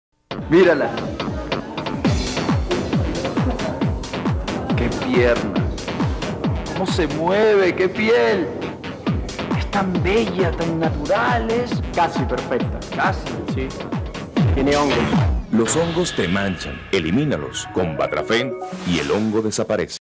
Batrafen Chica
The other of the 2 Freelance spots I directed for Batrafen anti-fungus cream, this time the guys check out the sexy girl doing spinning. This sexy girl has fungus somewhere. This was also lit by Ruben Belfort and shot on a Sony Beta SP camera, and it was edited on a a Avid Media Composer system with a film effect filter.